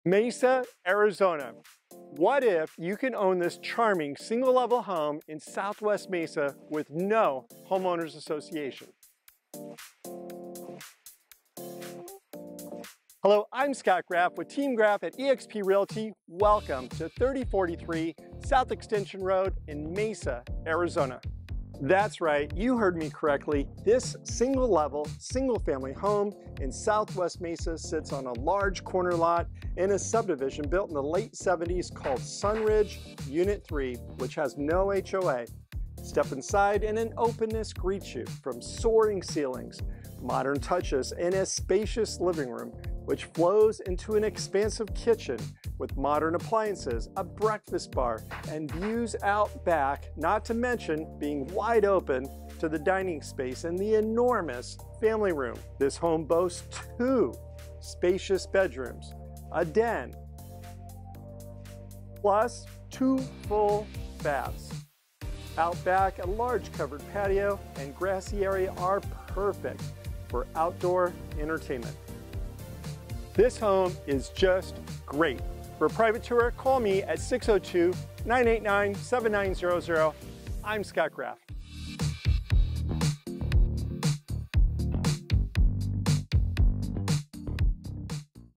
SOLD! 3043 South Extension Road Mesa, AZ 85210
SW Mesa Charmer! 🏠
👉 2 Bedrooms + Den
🎯 Living Room & Family Room
🐕‍🦺 Large Back Yard
🚀 NO HOA
$425,000
LIKE - Your "Likes" help more people like you find our videos!
FaceBook - TEAM GRAFF, Scott Graff
GET IN TOUCH WITH US:
Scott Graff - TEAM GRAFF at eXp Realty (Arizona Realtor)